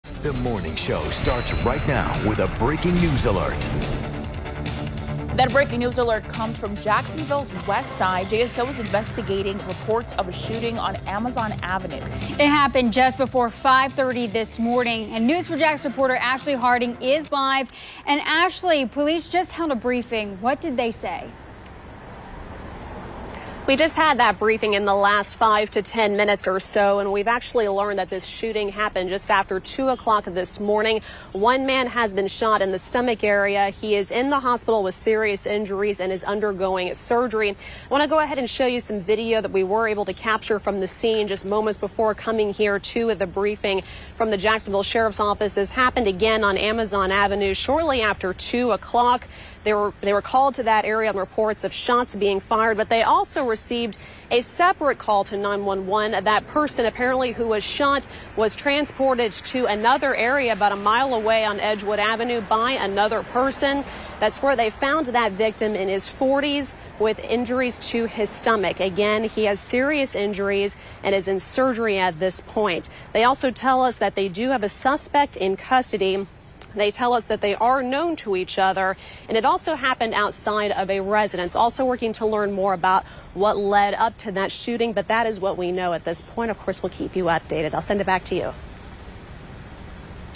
Police investigate shooting on Westside
A man was found shot in the stomach at the corner of 2200 Edgewood Ave. early Saturday morning, police say.